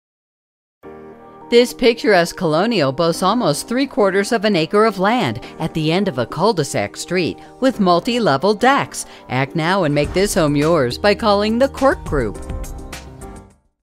26 Orchard Park Drive Reading, MA | ColdwellBankerHomes.com
26 Orchard Park Drive, Reading, MA

Offered for the first time in 50 years! Boasting almost three-quarters of an acre of land, this central entrance colonial located at the end of a cul-de-sac street on Reading’s coveted West Side presents a special opportunity. The home has been lovingly cared for and extensively renovated into a fou